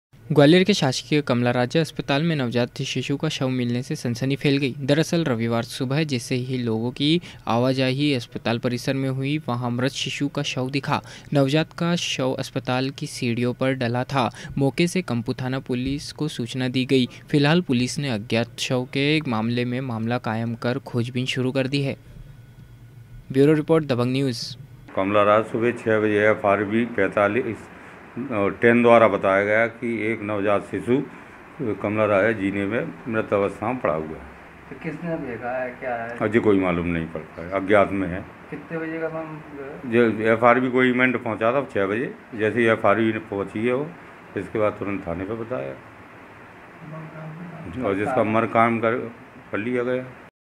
GWALIOR नवजात शिशु का शव मिलने से सनसनी फैल गई
ग्वालियर के शासकीय कमलाराजा अस्पताल में नवजात शिशु का शव मिलने से सनसनी फैल गई।दरअसल रविवार सुबह जैसे ही लोगो कीआवाजाही अस्पताल परिसर में हुई वहाँ मृत शिशु का शव दिखा। नवजात का शव अस्पताल की सीढ़ियों पर डला था। मौके से कम्पू थाना पुलिस को सूचना दी गयी। फिलहाल पुलिस ने अज्ञात शव के मामले में मामला कायम कर खोजबीन शुरू कर दी है।